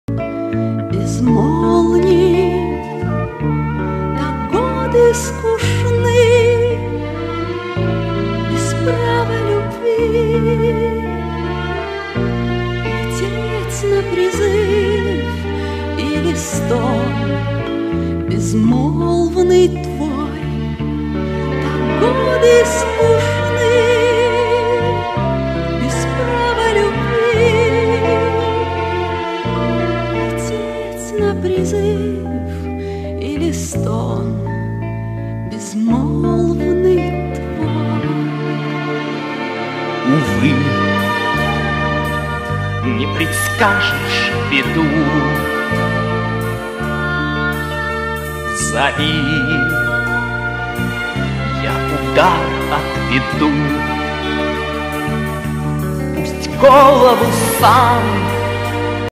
Татьяна Васильева
Татьяна Васильева - народная артистка россии, звезда театра сатиры. По прежнему в отличной творческой форме.